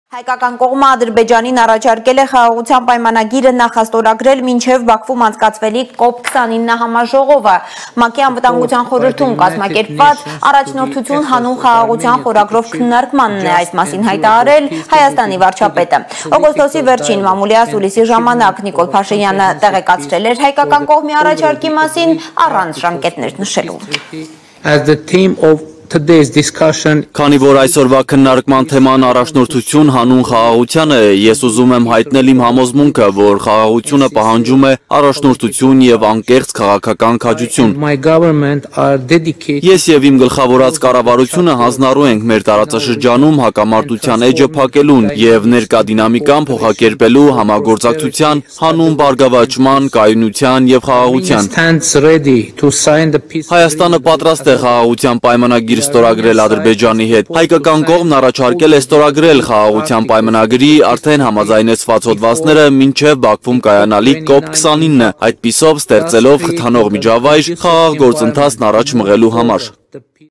Փաշինյանն ուզում է խաղաղության պայմանագիր ստորագրել մինչև Բաքվի COP29-ը
Հայկական կողմը Ադրբեջանին առաջարկել է խաղաղության պայմանագիրը նախաստորագրել մինչև Բաքվում անցկացվելիք COP 29-ը համաժողովը: ՄԱԿ-ի Անվտանգության խորհրդում կազմակերպված «Առաջնորդություն հանուն խաղաղության» խորագրով քննարկմանն է այս մասին հայտարարել Հայաստանի վարչապետը: